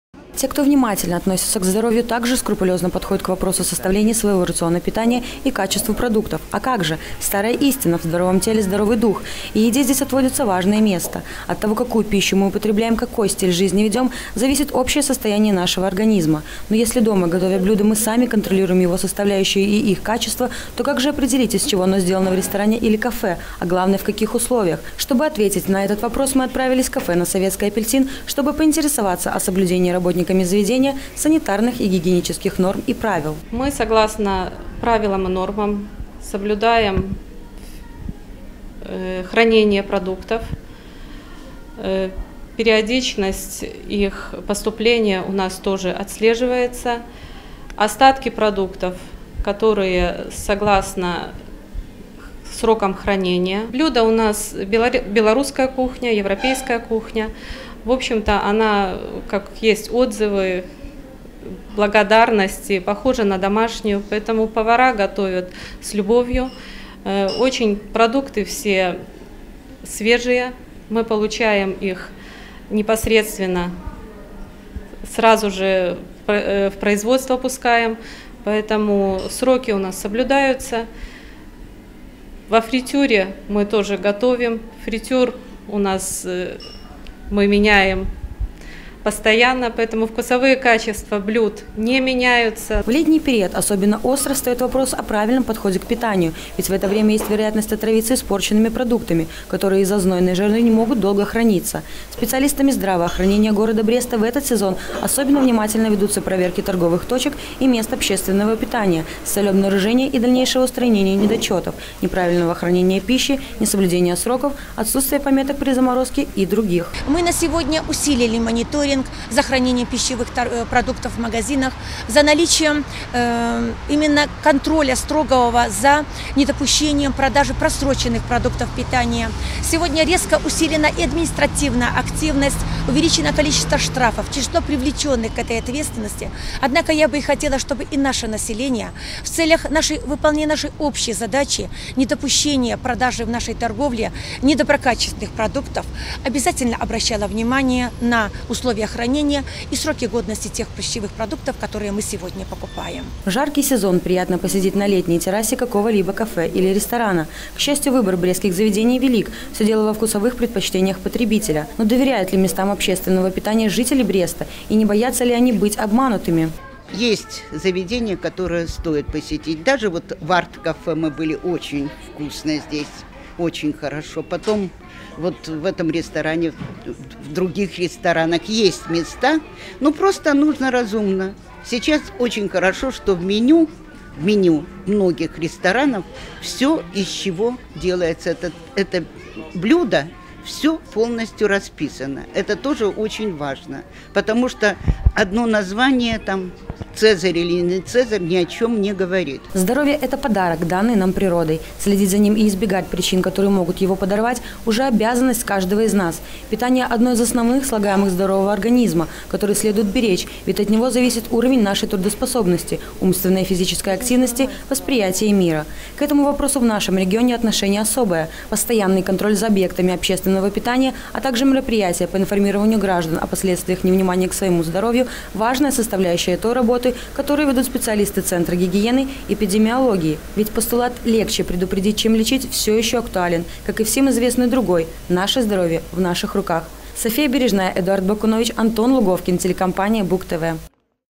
2014-08-20 г. Брест Телекомпания Буг-ТВ. День здорового питания на Брестчине.
Мы - есть то, что мы едим. И с этим не поспоришь. Именно здоровое питание –залог физической активности, красивого тела, хорошего настроя и самочувствия.   Кто-то внимательно следит за своим рационом и качеством пищи, кто-то обращается за советами к специалистам, а другие  вовсе не задумывается над тем, что   поесть, когда приходит время завтрака, обеда или ужина. Как раз этим людям и стоит  задуматься и взглянуть на ситуацию с другой стороны.
 Ежегодно во всём мире проходит День здорового питания,  в Бресте сотрудниками  учреждений здравоохранения в этот раз  была проведена большая работа по информированию граждан об организации здорового рациона, привлекалось внимание и к употреблению пищи в местах общественного питания, сделан был акцент и на правильное хранение.